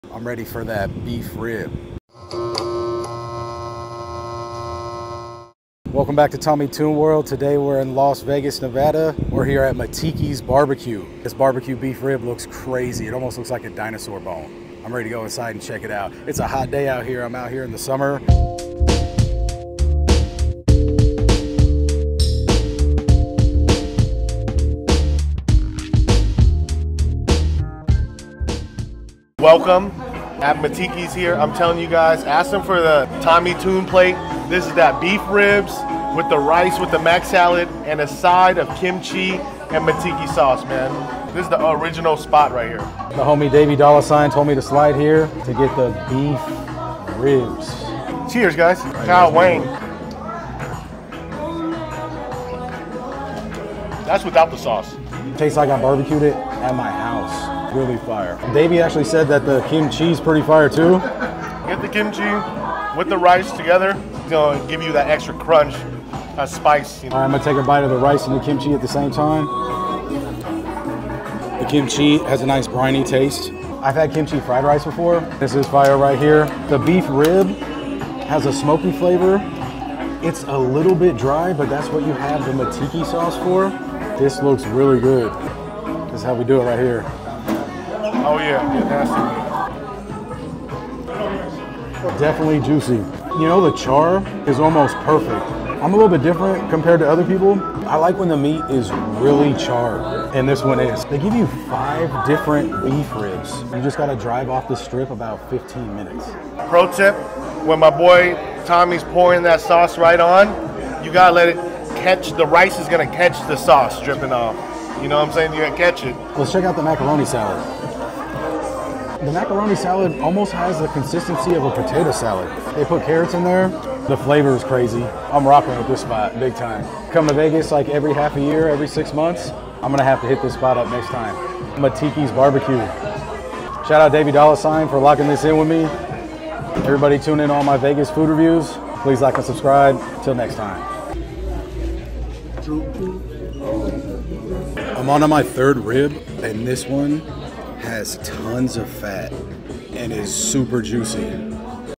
BEEF RIB at MATIKI ISLAND Hawaiian BBQ | Las Vegas 2024
Check out the BEEF RIBS from Matiki Island BBQ in Las Vegas, Nevada. Do you think you will try them?

0:00 intro
0:20 beef rib
0:58 kimchi
1:15 sauce
1:50 pro tip
1:59 macaroni salad
2:08 outro
2:22 eating

SPECIAL GUEST:

Davey Dolla $ign

Merch!